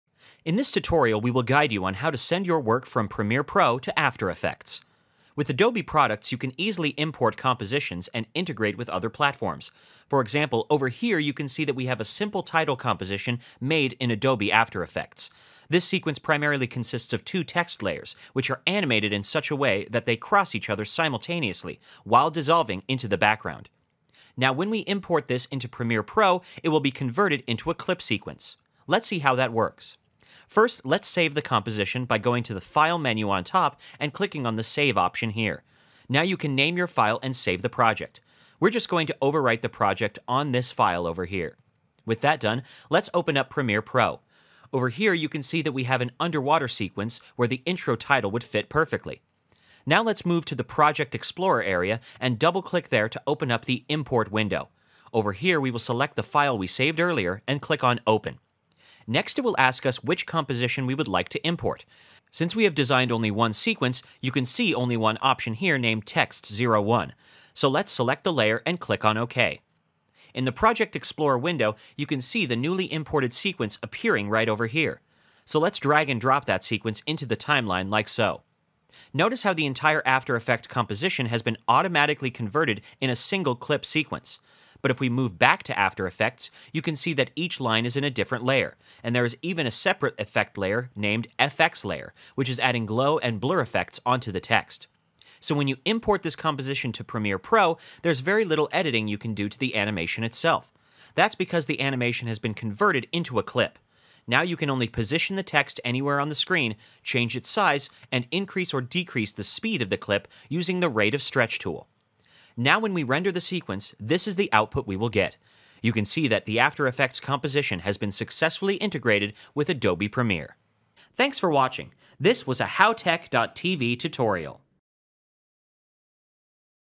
How to Send Work from Premiere Pro to After Effects
In this tutorial, we will guide you on how to send your work from after effects to premiere.

With adobe products, you can easily import compositions and integrate with other platforms. For example, in this tutorial we will be working with a simple title composition made in Adobe After Effects. The sequence will primarily consist of two text layers which will be animated in such a way that they cross each other simultaneously, while dissolving into the background.

When we import in adobe premiere, it will be converted into a clip sequence. Let's see how that works.

Step 1 -- Saving in After Effects
Let's first save the composition by going to the "File" menu and clicking on the "Save" option.

Step 2 -- Naming your file
Now you can name your file and save the project. For this tutorial, we will just overwrite the project on this file over here.

Step 3 -- Opening After Effects file in Premiere
With that done, let's open up Premiere Pro. For the purpose of this tutorial, we will be working with an underwater sequence where the intro title would fit perfectly.
Now let's move to the project explorer area and double click anywhere within the explorer. This will open up the "Import" Window. Over here, we will select the file we saved earlier and click on "Open" to import in adobe premiere.

Step 4 -- Selecting the composition
Once the composition has been imported from after effects to premiere, it will ask us which composition we would like to import. Since we have designed only one sequence for this tutorial, you can see only one option here named "Text 01". Otherwise, you will get a list of all the compositions present within the file and you can individually select which composition is to be imported. Moving on, let's select the layer and click on "Ok".

Step 5 -- Adding after effects to the clip Sequence
In the project explorer window, you can see the newly imported sequence appearing right over here. So let's drag and drop that sequence into the timeline like so.
Notice how the entire After Effect composition has been automatically converted in a single clip sequence.  Because of this, there is very little editing you can do to the animation itself. Now you can only position the text anywhere on the screen, change its position and increase or decrease the speed of the clip using the rate of stretch tool.

With that done, we have successfully added the After Effects and integrated it with Adobe Premiere.